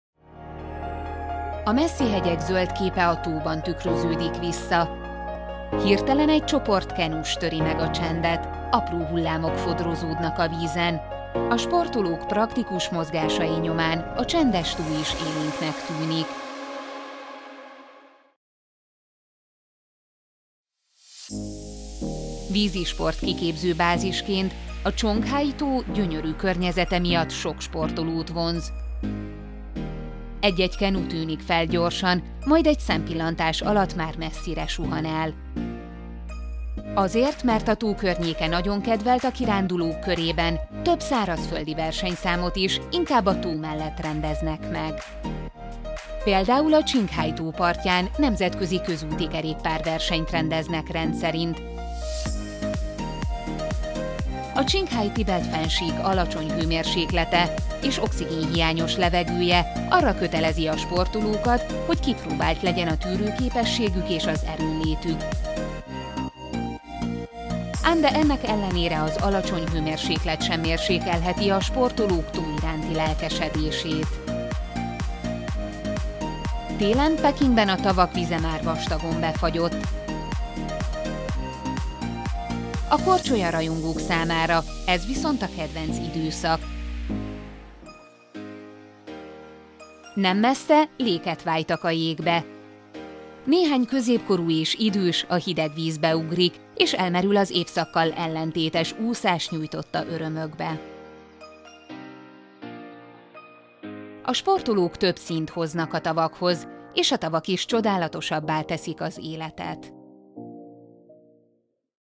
Sportoljunk tavaknál! - Útjelzők a Selyemúton
A messzi hegyek zöld képe a tóban tükröződik vissza. Hirtelen egy csoport kenus töri meg a csendet, apró hullámok fodrozódnak a vízen. A sportolók praktikus mozgásai nyomán a csendes tó is élénknek tűnik. Vízi sport kiképző bázisként a Qinghai-tó gyönyörű környezete miatt sok sportolót vonz. Egy-egy kenu tűnik fel gyorsan, majd egy szempillantás alatt már messzire suhan el. Azért, mert a tó környéke nagyon kedvelt a kirándulók körében, több szárazföldi versenyszámot is inkább a tó mellett rendeznek meg. Például a Qinghai-tó partján nemzetközi közúti kerékpárversenyt rendeznek rendszerint. A Qinghai-Tibeti-fennsík alacsony hőmérséklete és oxigénhiányos levegője arra kötelezi a sportolókat, hogy kipróbált legyen a tűrőképességük és az erőnlétük. Ámde ennek ellenére az alacsony hőmérséklet sem mérsékelheti a sportolók tó iránti lelkesedését. Télen Pekingben a tavak vize már vastagon befagyott, a korcsolyarajongók számára ez viszont a kedvenc időszak. Nem messze léket vájtak a jégbe. Néhány középkorú és idős a hideg vízbe ugrik, és elmerül az évszakkal ellentétes úszás nyújtotta örömökbe. A sportolók több színt hoznak a tavakhoz, és a tavak is csodálatosabbá teszik az életet.